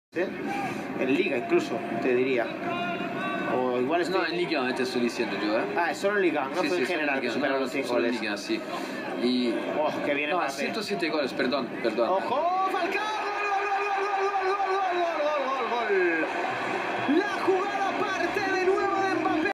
GOL DE FALCAO VS TOULOUSE. Tempo 2017/18.
El colombiano marcó en el empate parcial de Mónaco contra Toulouse por 2-2.